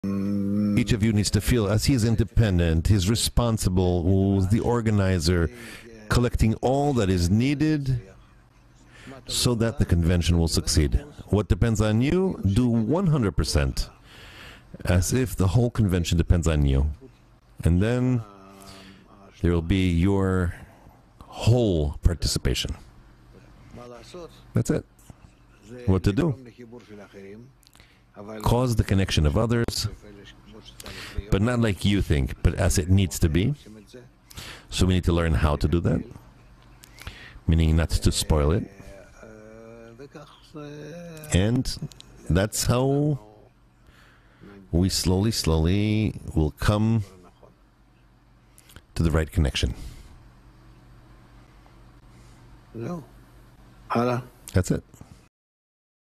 What do I have to do for the Congress?
Lesson on the topic of "Love Covers All Crimes" (Preparation for convention in Bulgaria)